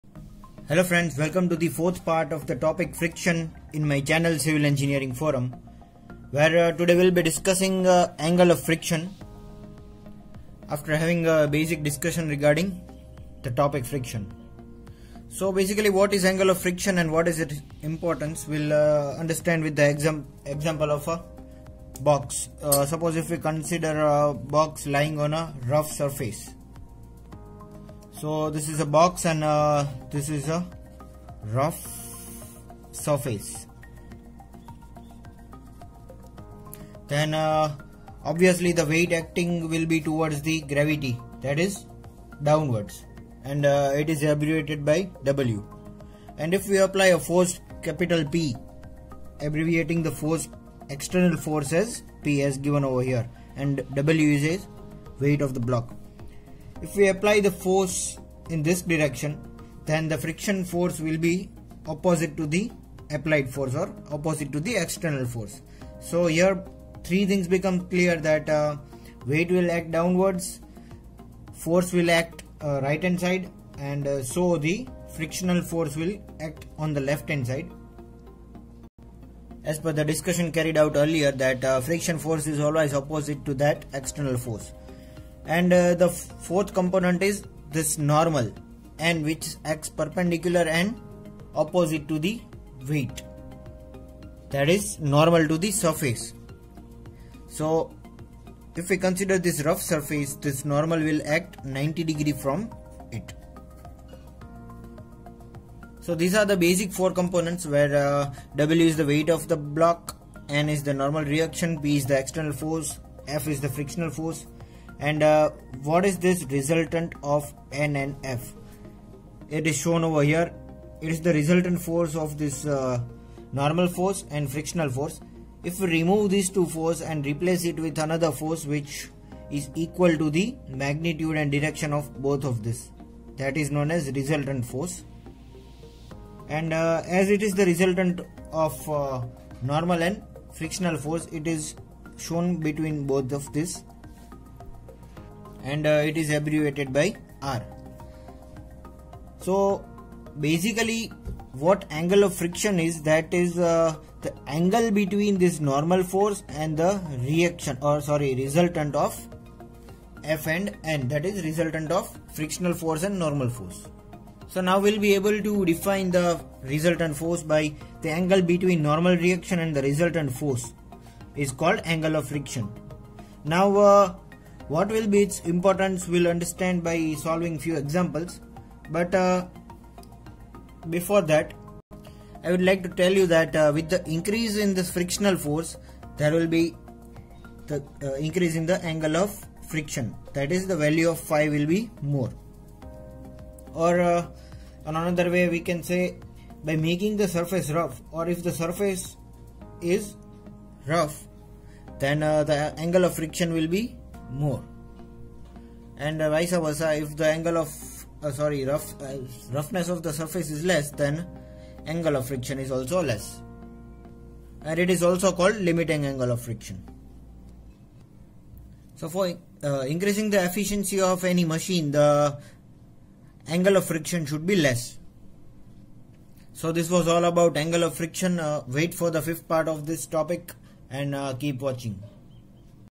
What is Angle of Friction | Part 4
In this lecture, I have explained the angle of friction in detail with its use and figure. Components of the forces/weight acting are also explained here.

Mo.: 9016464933